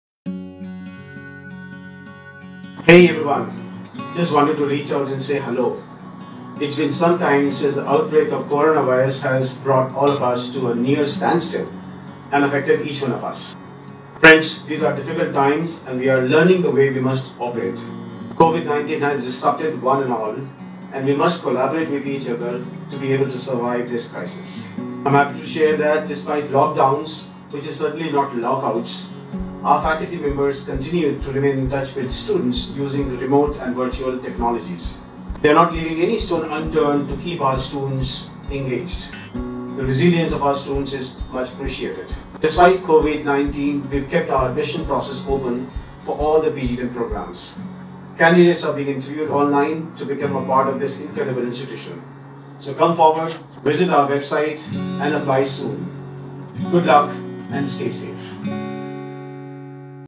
Message from Our Director Dr. Bigyan Verma
During this pandemic situation all over the world, what we all are lacking with, is motivation.
So here is our Director Dr. Bigyan Verma, motivating us through his msg and also how SIESCOMS is over coming all these hurdles and is standing functional.
Let's hear what he has got to share!